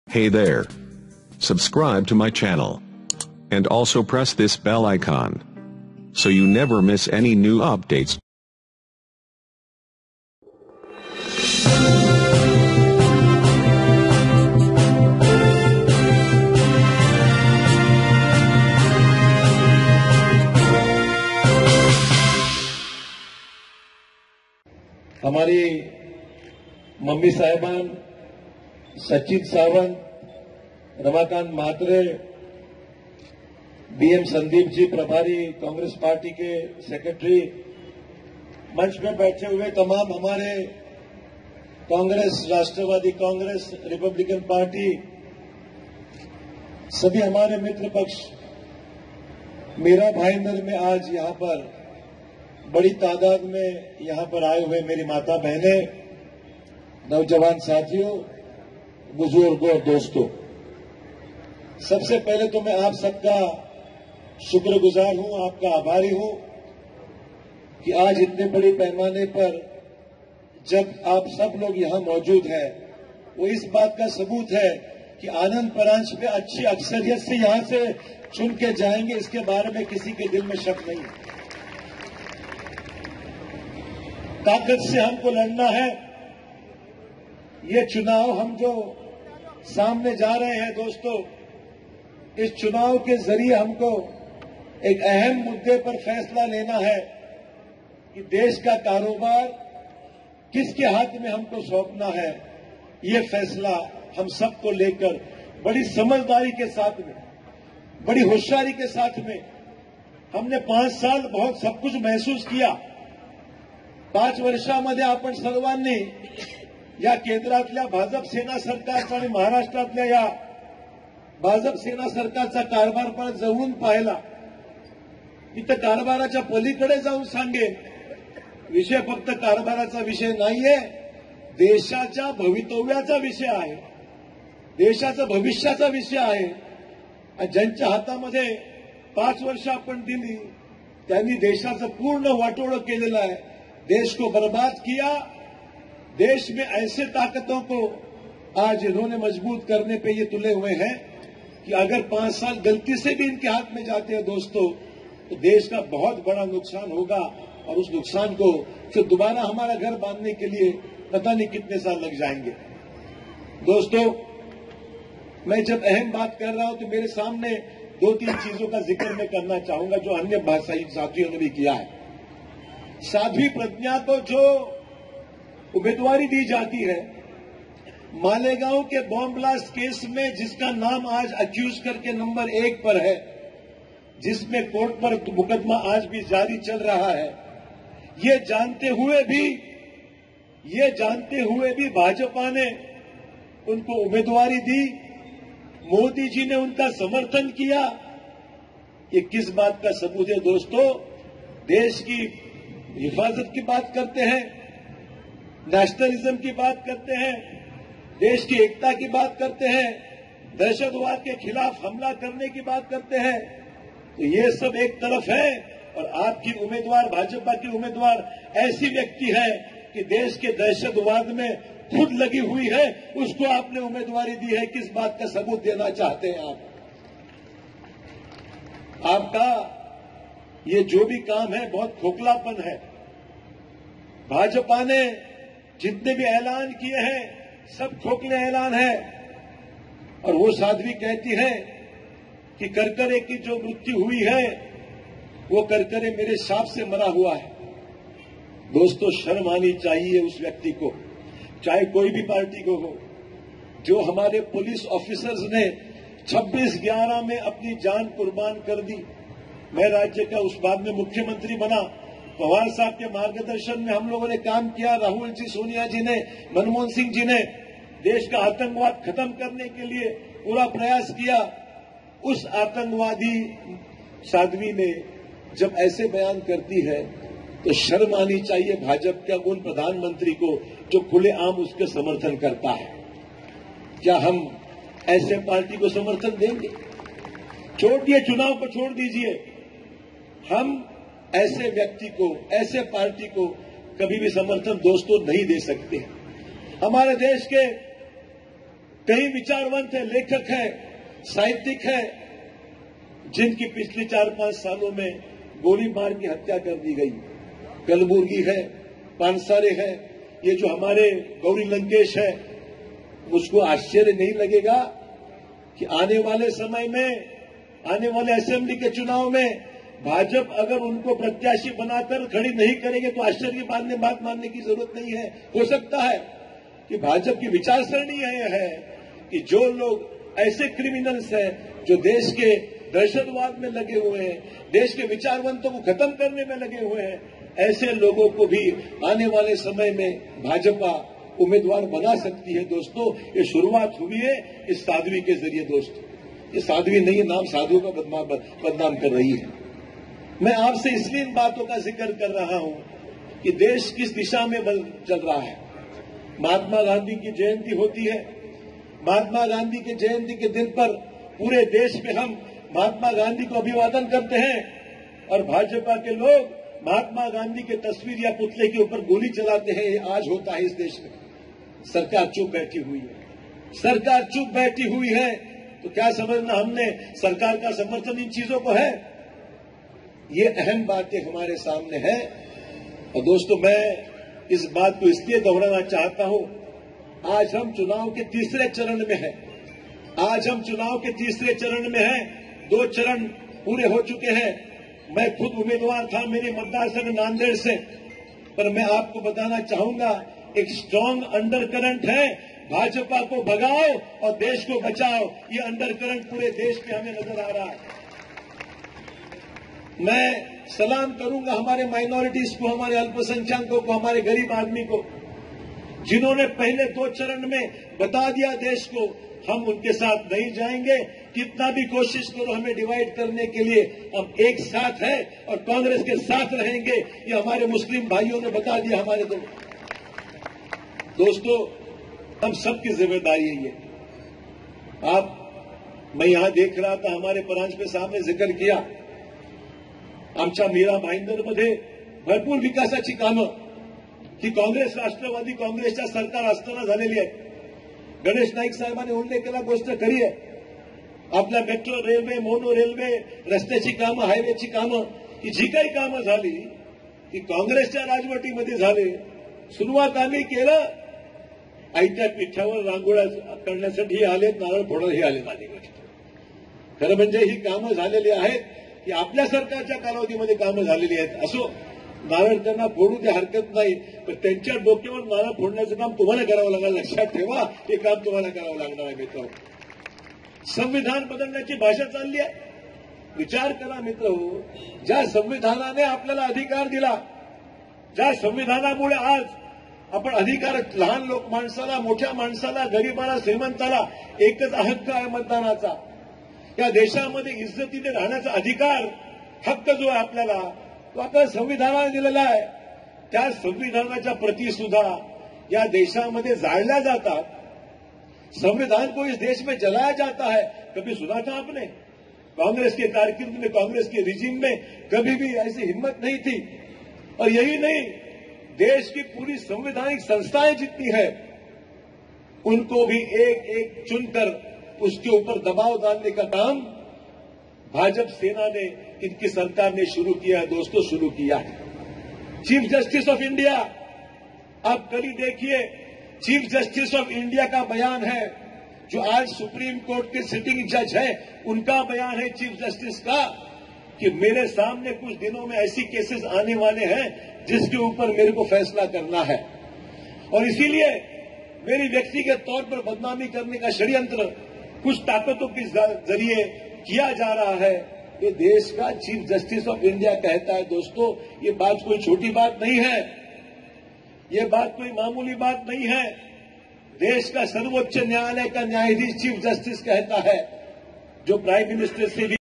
साध्वी प्रज्ञा सिंह ठाकुर को लोकसभा चुनाव में उतारकर क्या साबित करना चाहती है भाजपा - अशोक चव्हाण
Ashok Chavan in Mira Bhayander

राष्ट्रवादी कांग्रेस और कांग्रेस की सभा का आयोजन मीरा भाईंदर के मेड़तिया ग्राउंड में किया गया।

इस सभा में हजारों की संख्या में मीरा भाईंदर के नागरिक मौजूद थे।

राष्ट्रवादी कांग्रेस के वरिष्ठ नेता शरद पवार, कांग्रेस के अशोक चव्हाण, गणेश नाईक, लोकसभा उम्मीदवार आनंद परांजपे, डॉ. संजीव नाईक, मुज़फ्फर हुसैन तथा एनसीपी और कांग्रेस के कई अन्य कार्यकर्ता नगरसेवक नगरसेविका उपस्तिथ थे .

YND NEWS